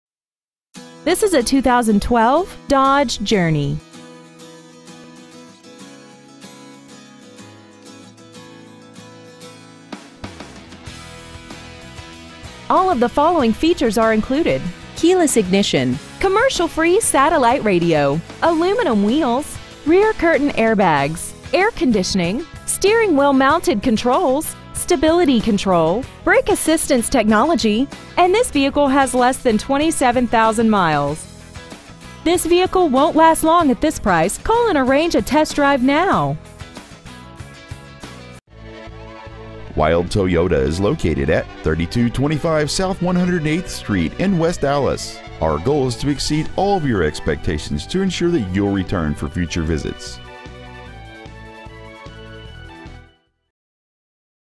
2012 Dodge Journey Milwaukee WI
We have been honored to serve the West Allis WI area , we promise that your experience at our dealership will exceed your expectations ! Year : 2012 Make : Dodge Model : Journey Engine : V6 Cylinder Engine Trans . : 4-Speed A/T Exterior : Pearl White Tri-coat Miles : 26,660 Interior : Black Stock : P65658 Wilde Toyota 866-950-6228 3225 South 108th St. West Allis , WI 53227